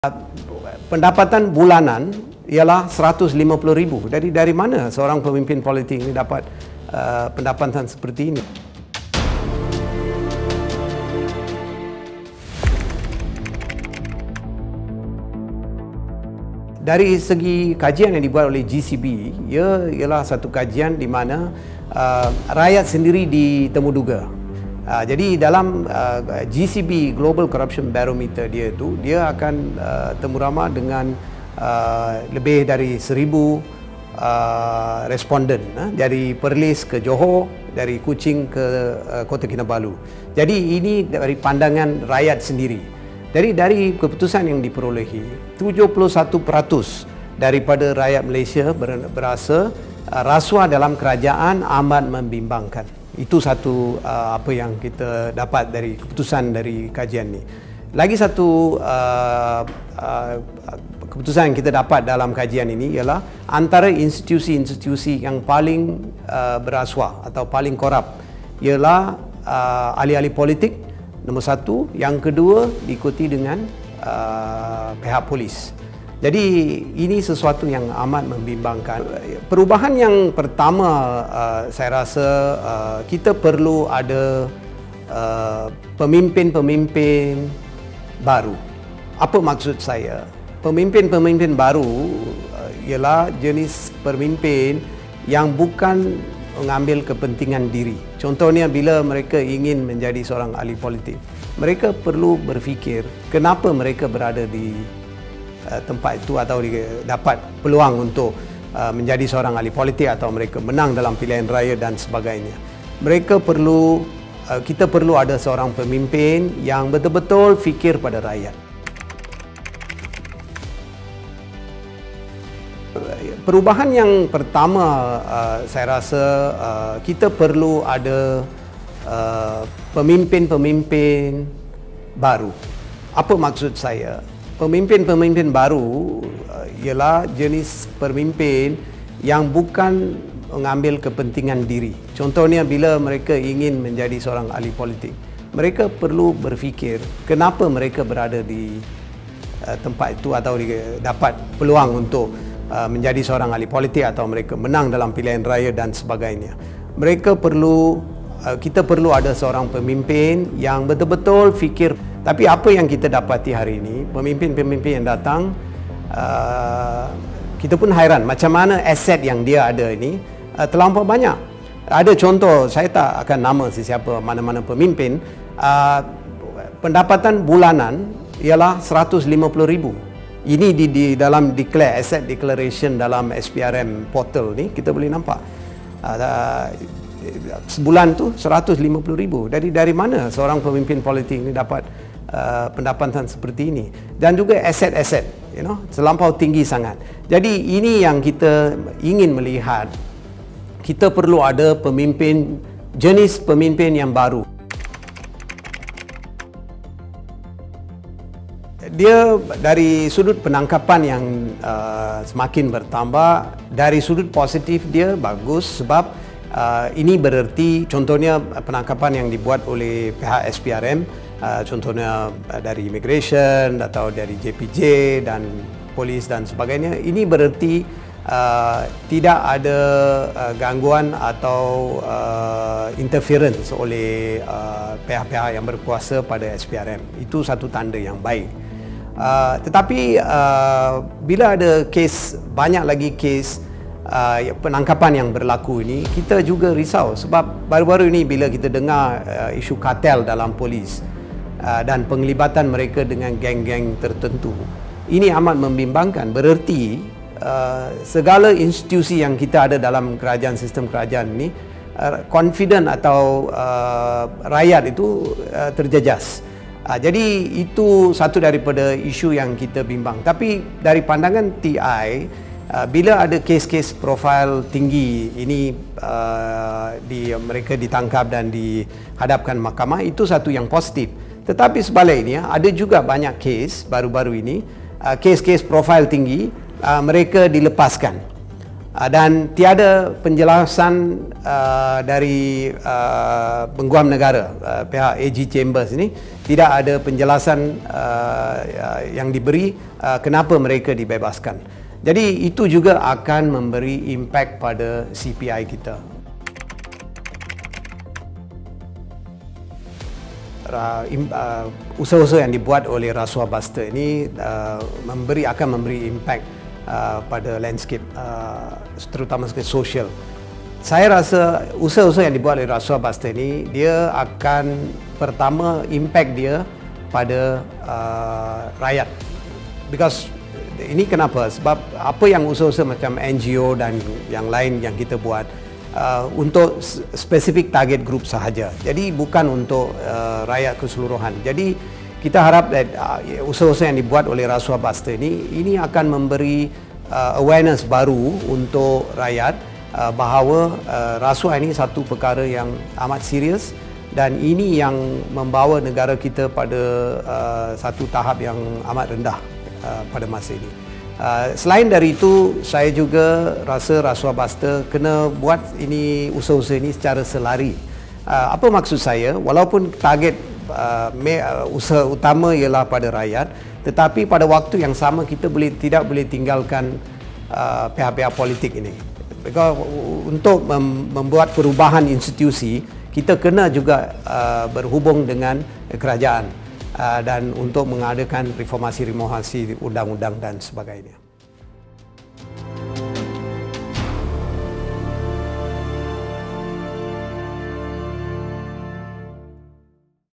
Negara perlu ada pemimpin baharu
Bukan mudah untuk menjadi seorang pemimpin yang berjiwa rakyat dalam mentadbir negara.

Negara perlu melahirkan pemimpin baru yang telus dan tidak pentingkan diri bagi menjadikan Malaysia bersih dari wabak rasuah.

Ikuti Kapsul Rasuah Busters bersama Presiden Transparency International Malaysia (TI-M), DR Muhammad Mohan berhubung pembanterasan rasuah dalam kalangan pemimpin.